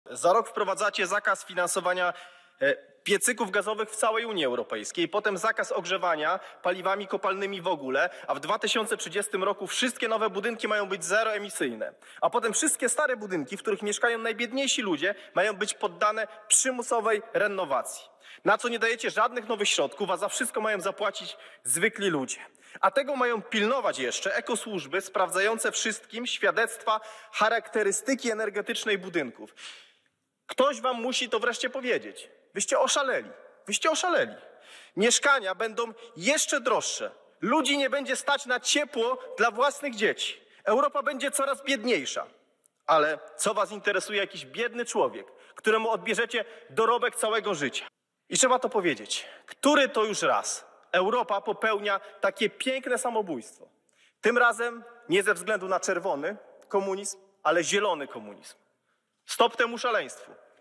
Zapłacą za to najbiedniejsi!
Już za chwilę najbiedniejsi ludzie odczują co to znaczy uśmiechnięta Unia Europejska - kiedy ta przyjdzie zabrać im pieniądze za niewłaściwe ogrzewanie domu.
Najbiedniejszym odbierze się możliwość opalania paliwami kopalnymi, wytworzy sztuczne podatki i dopłaty za emisję.

Patryk Jaki w emocjonalnym przemówieniu w Europarlamencie